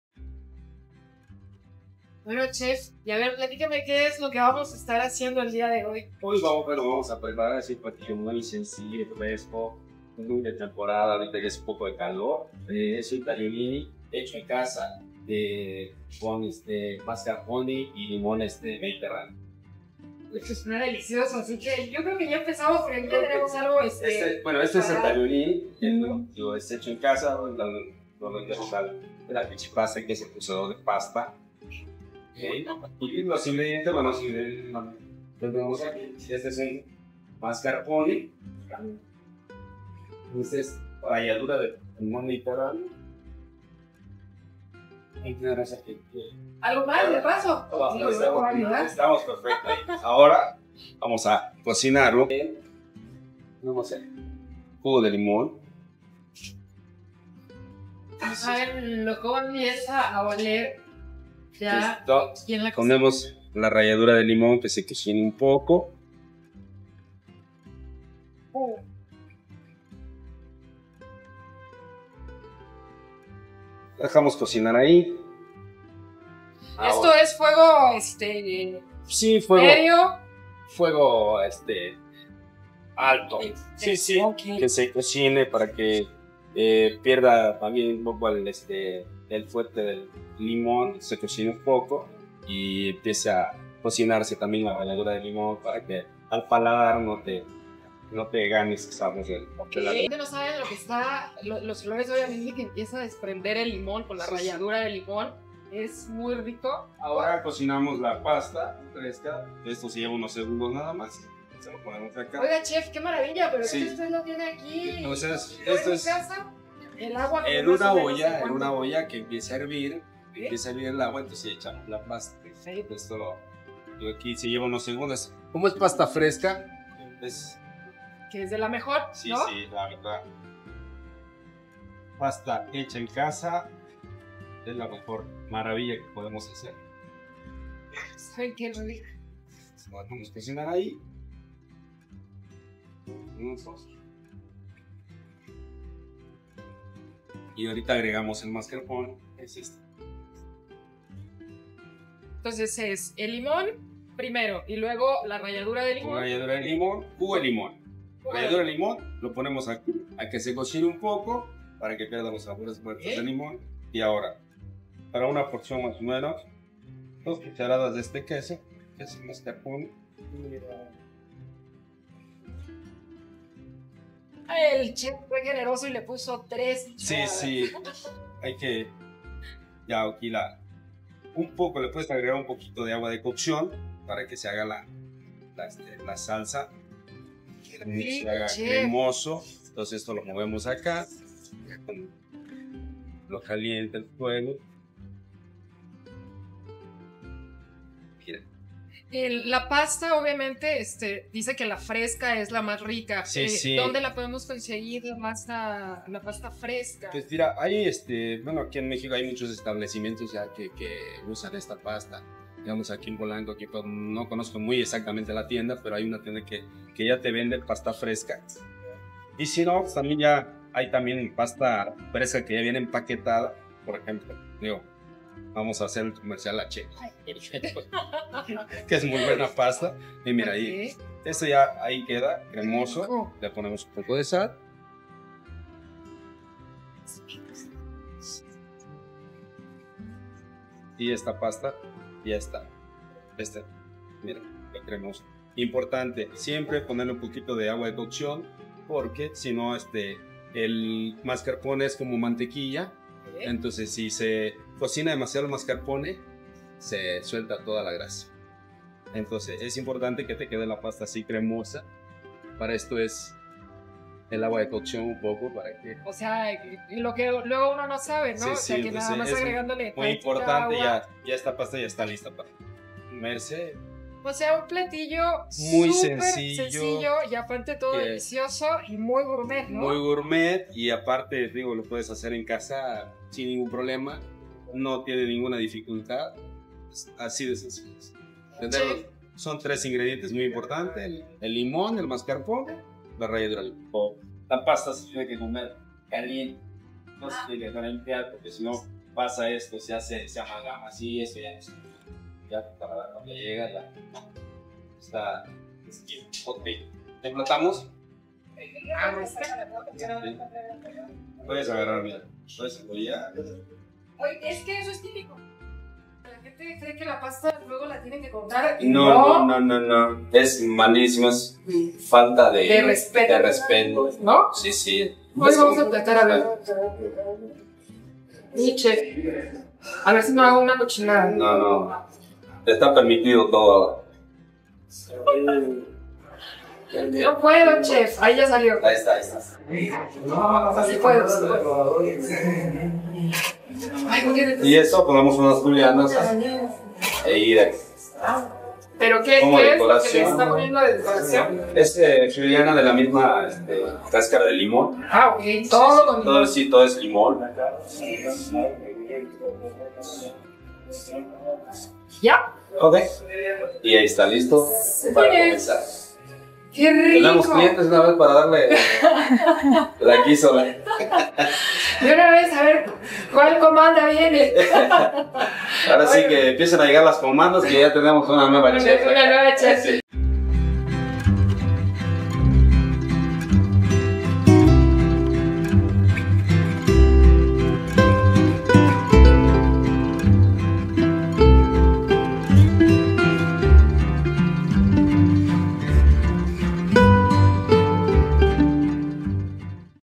LA RECETA DE ESTA PASTA CON MASCARPONE UNA DELICIA
Los tagliolini o taglierini son una variedad de pasta con huevo de las más finas, típica de la cocina italiana. Es una pasta de cocción rápida, que combina bien con salsas ligeras, aderezos delicados o incluso en caldo.
La prendimos hacer en uno de los restaurantes de la alta cocina italiana.
Te compartimos los ingredientes que son muy pocos.

INGREDIENTES:
Tagliolini
Jugo de limón mediterráneo
Ralladura de limón mediterráneo
Mascarpone
Sal
Agua de cocción de la pasta